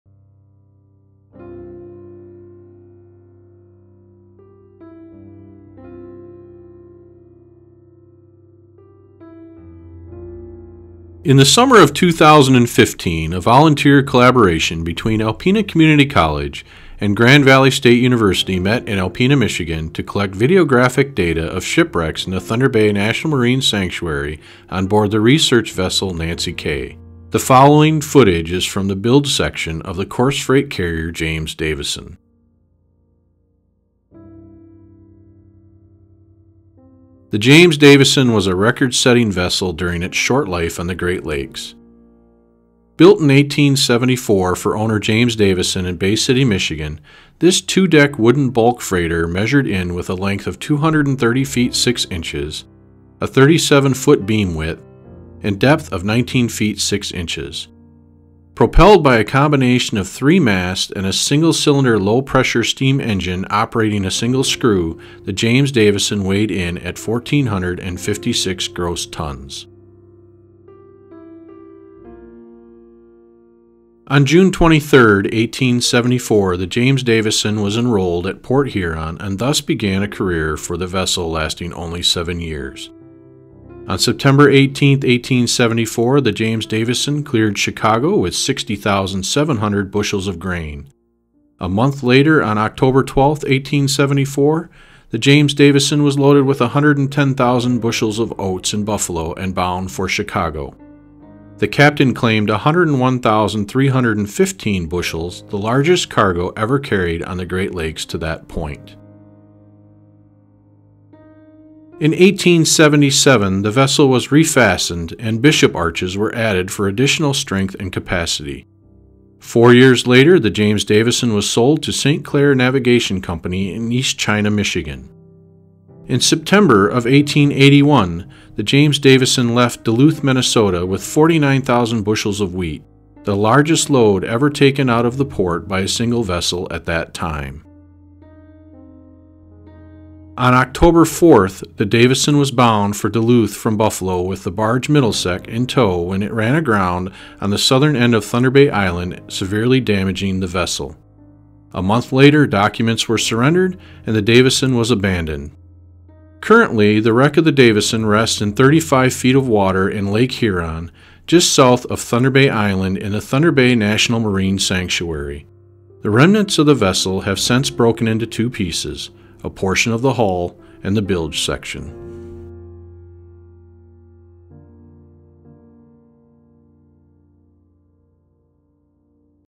Your Earth Is Blue: The Wreck of the James Davidson
After a relatively short career as a freighter -- in which it carried what was then the largest load ever on the Great Lakes -- the James Davidson ran aground on October 4, 1883 in what is now Thunder Bay National Marine Sanctuary. Last summer, a team of volunteers from Alpena Community College and Grand Valley State University used an ROV to investigate the wreck of the Davidson. Check out what they found! Special thanks to Tim Parsell for sharing this video with us.

Do you have video or photos that you've taken in a national marine sanctuary?